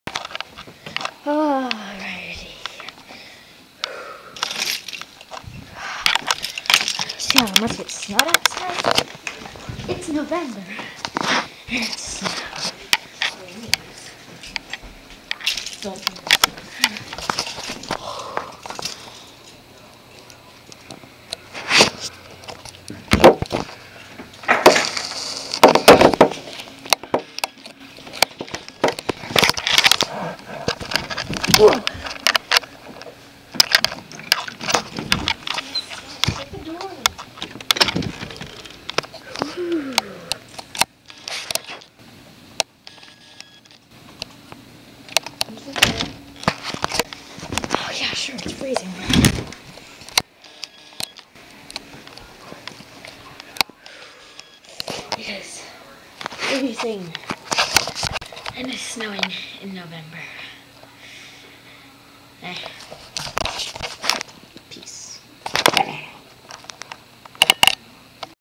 snow in November
it snowed in November!!!and it is just freezing cold now!